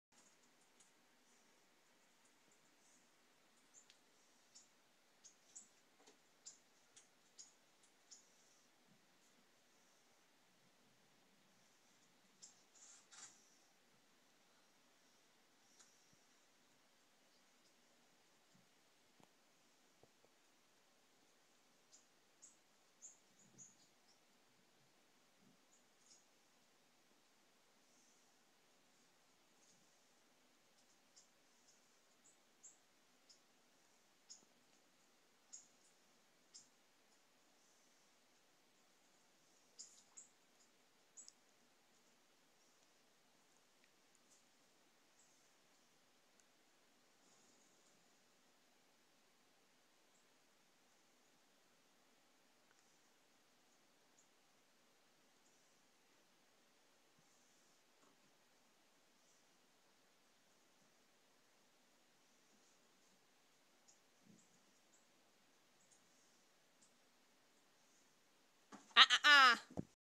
Cats watching on big tv
Thought it would be funny to watch my cats watch the tv until they try to paw the screen. Yikes! :) LoL